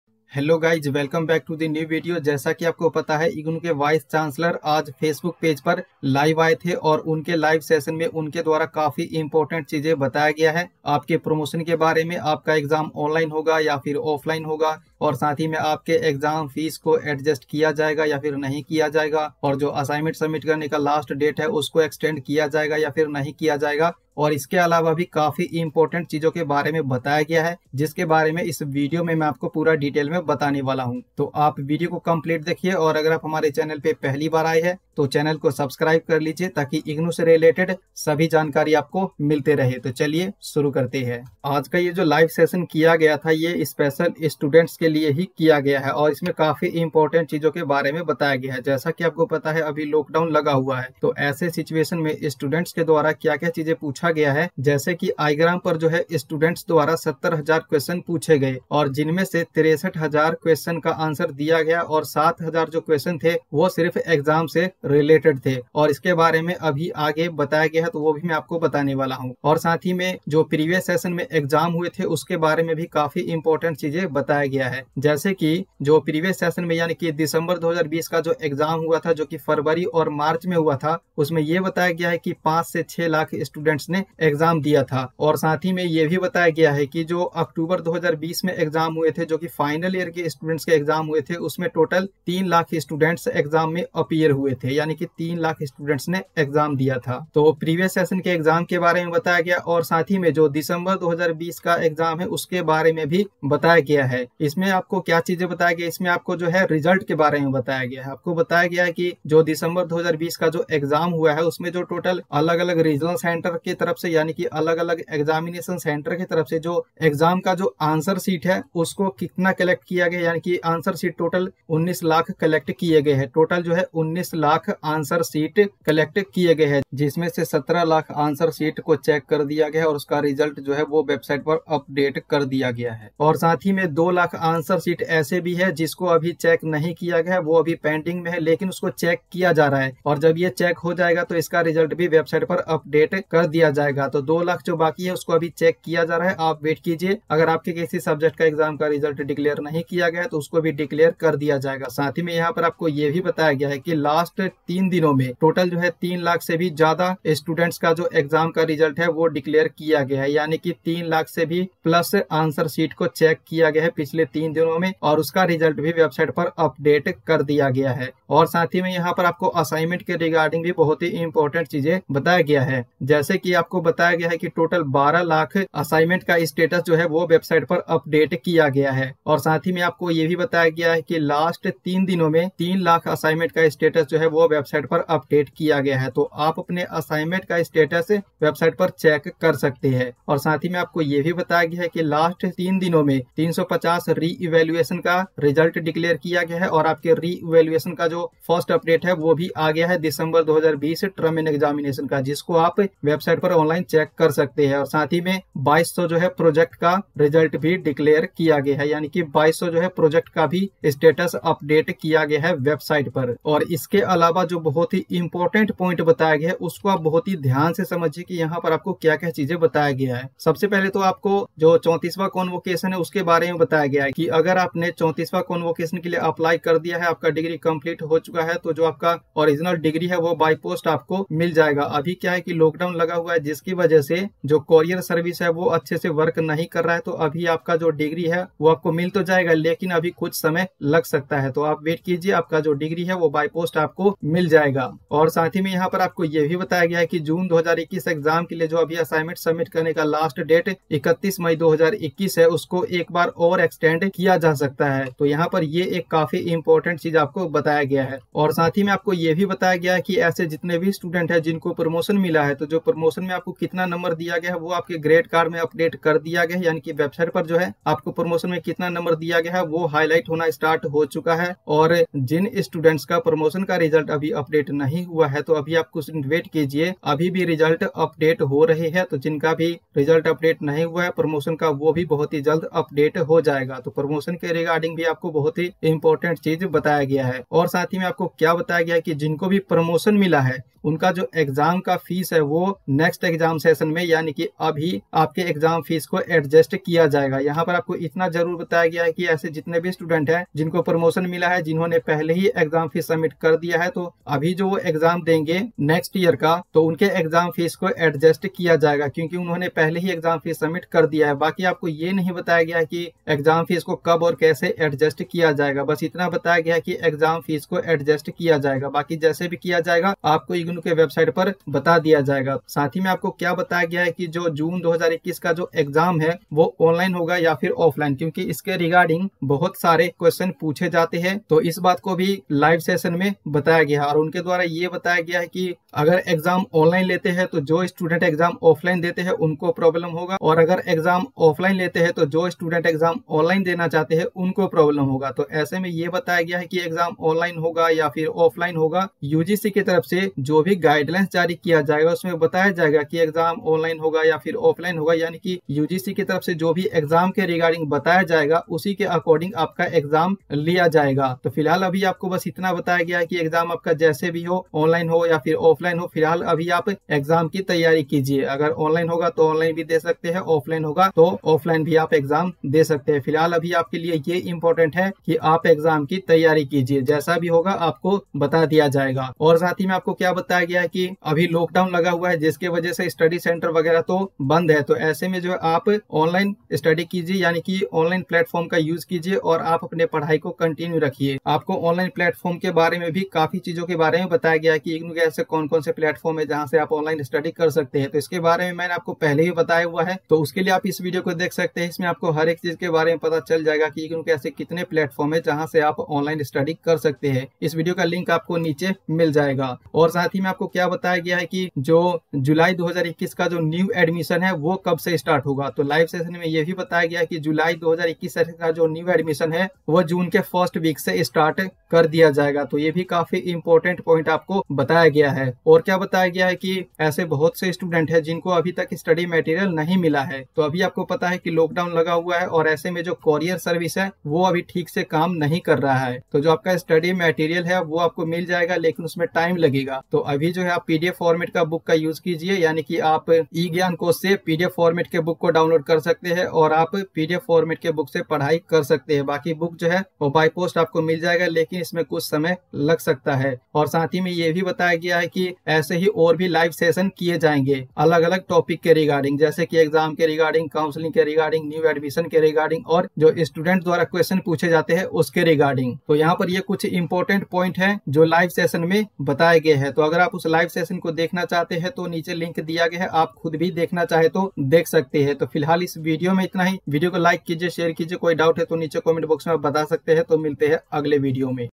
सभी के लिए Good News | Promotion & June 2021 Exam New Update | Address of the VC to the All Students
Note📌
All videos on this YouTube channel are non-promotional & make it only for information and educational purpose.